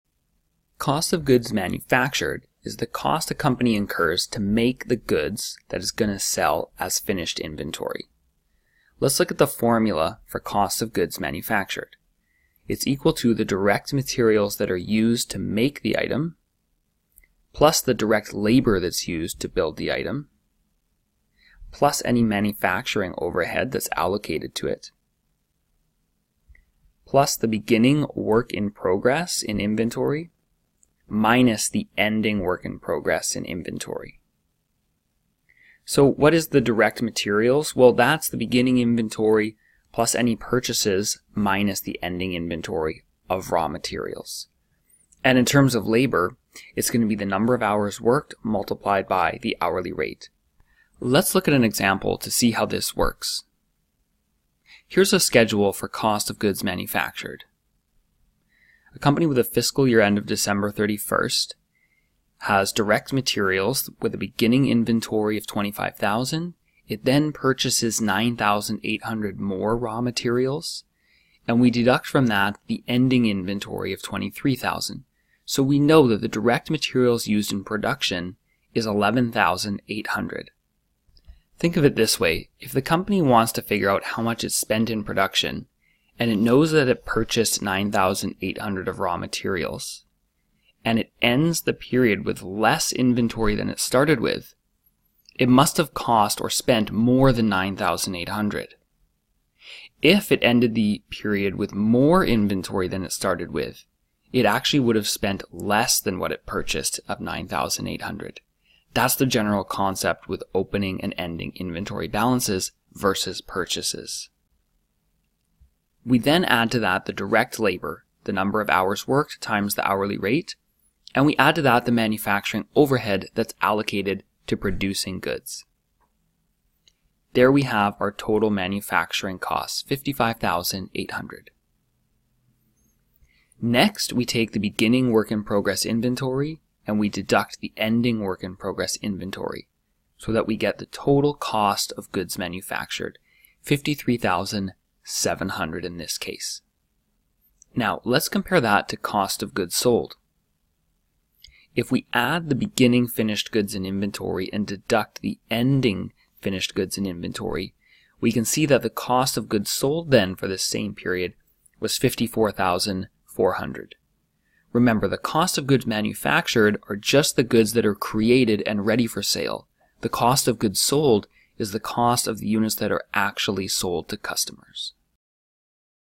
How to Calculate Cost of Goods Manufactured [COGM]
Cost of Goods Manufactured, also known as COGM, is a term used in managerial accounting that refers to a schedule or statement that shows the total production costs for a company during a specific period of time.